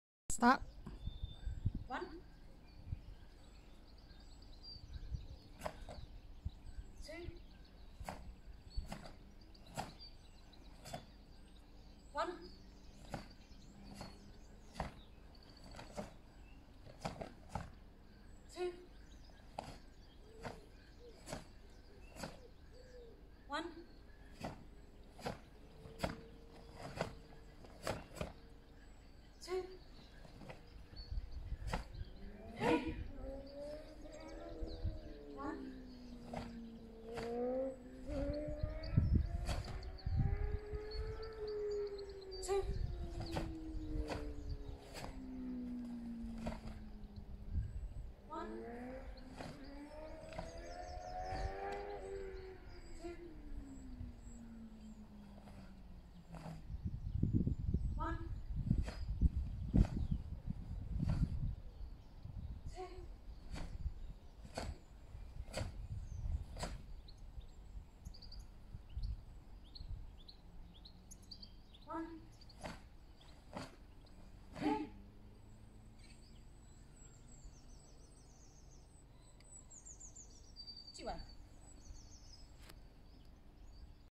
Tang Sou Dao : Luohan 6
Senior form performed by Master Loke and Master Knight